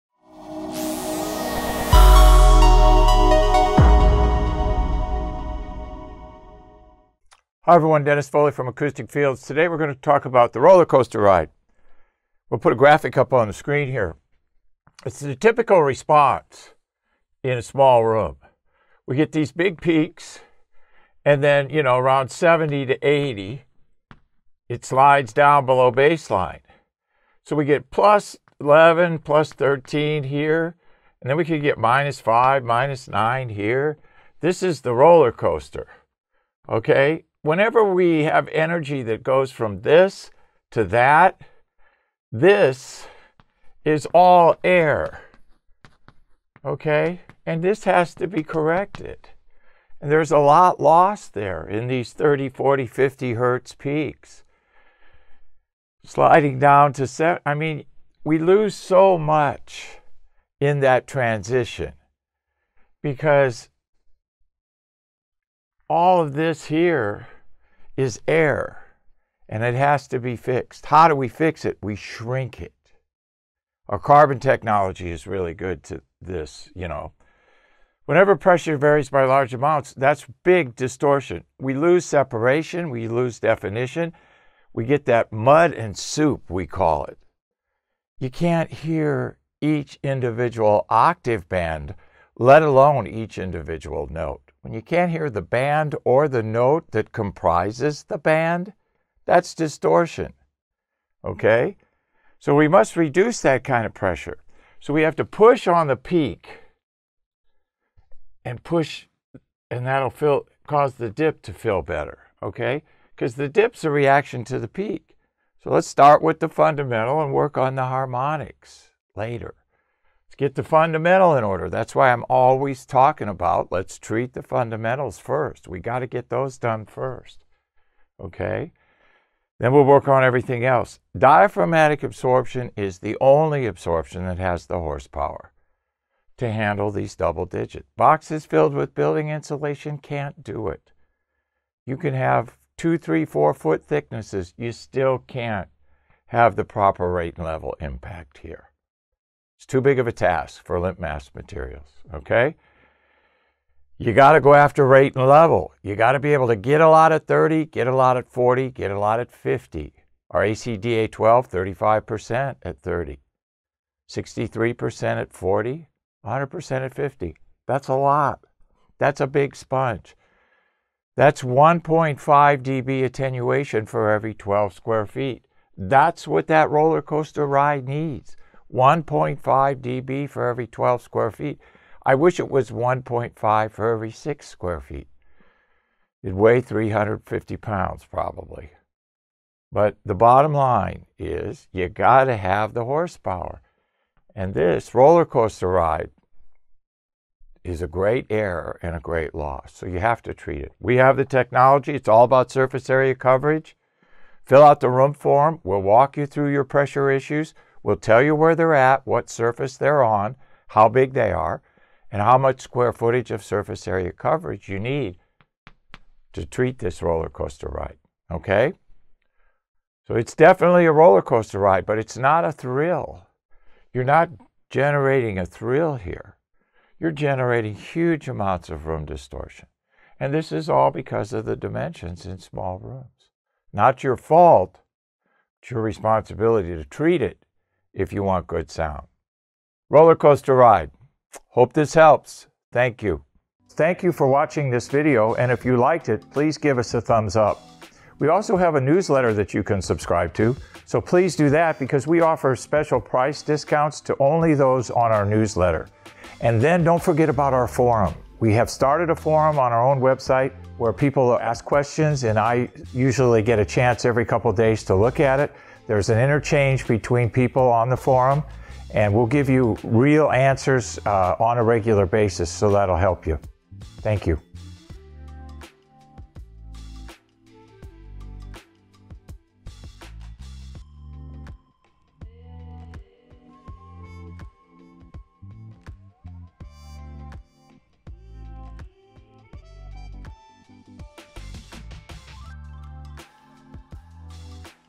The Roller Coaster Ride
- - In this video we're going to talk about what we call the "roller coaster ride" and why it is important to absorb low frequency energy at 30,40 and 50Hz. Watch the video to find out more!